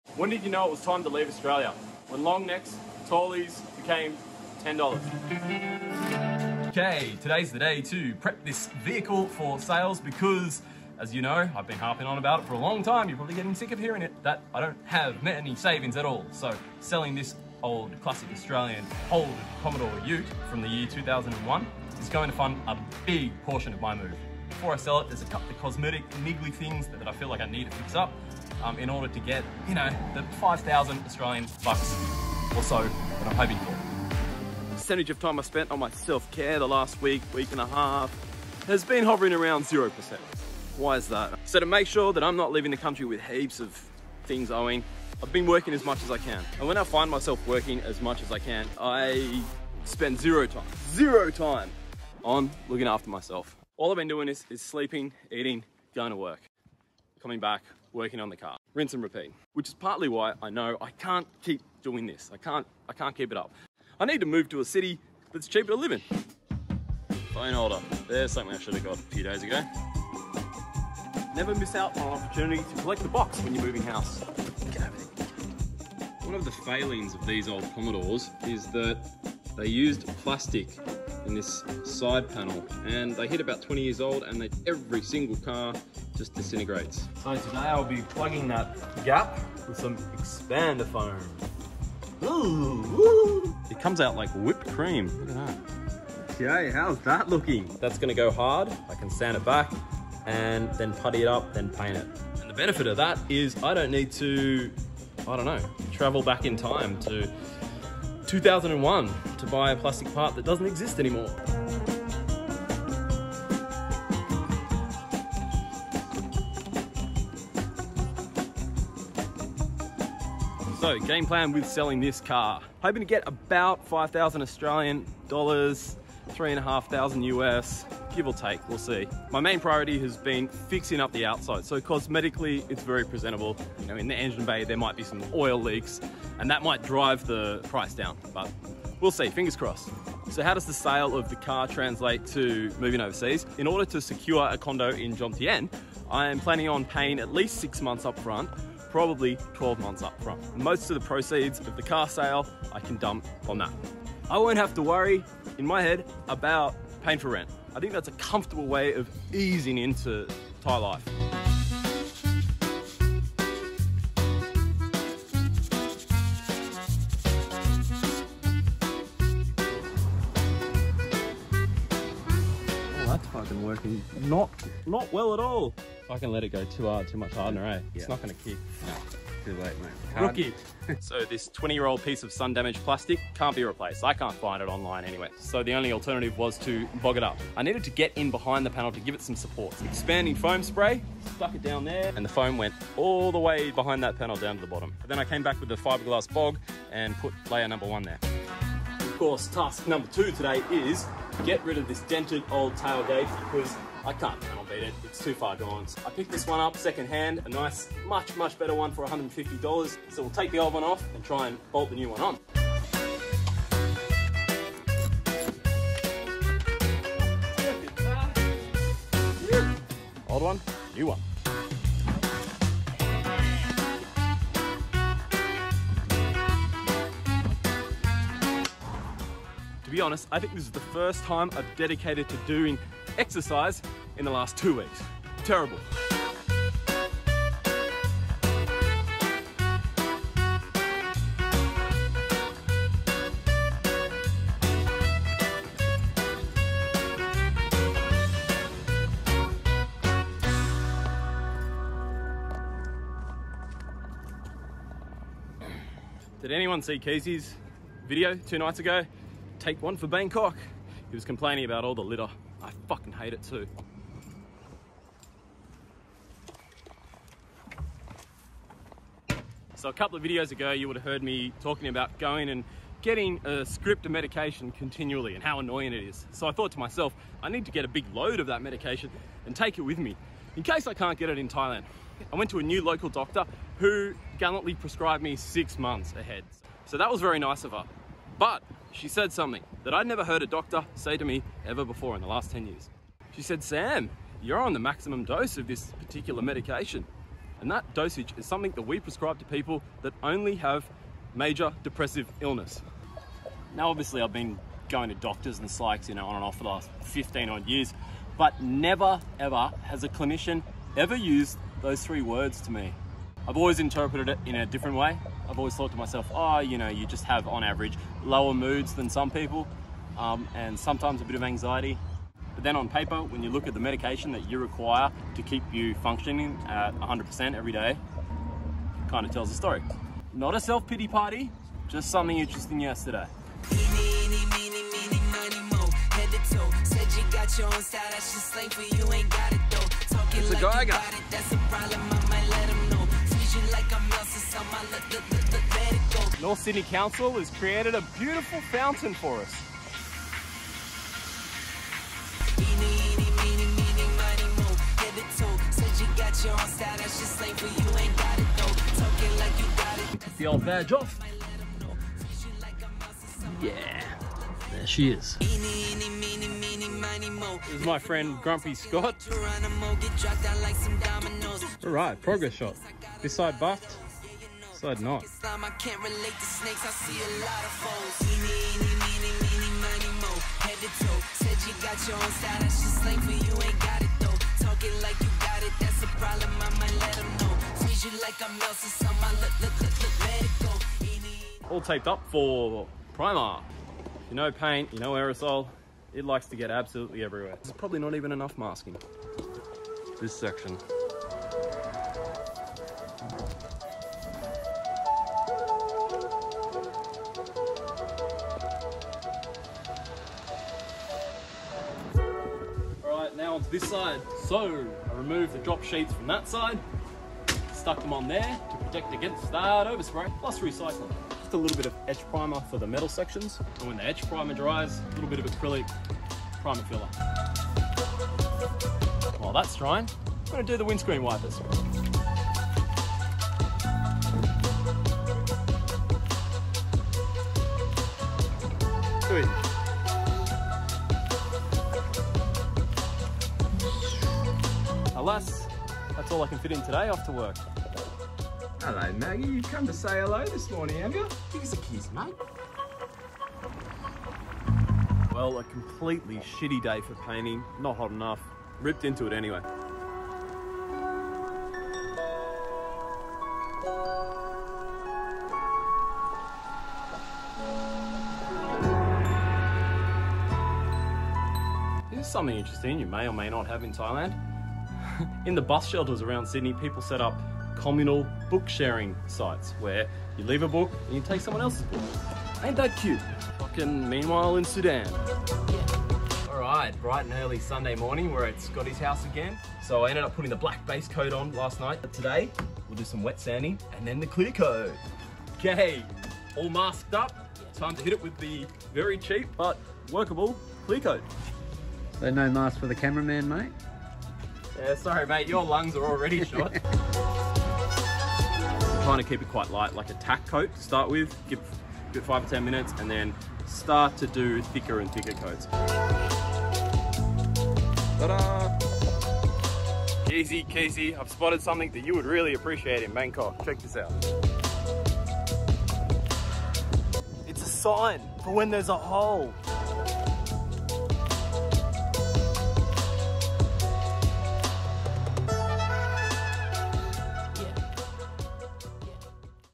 MOVING to THAILAND - sell CAR to RENT Apartment?
In moving to Pattaya, I spend 1.5weeks of hell fixing / sanding / painting car to sell in order to sell it to pay for 6 months up front rental costs at Jomtien Beach, Thailand.

🎥 Favourite Vids!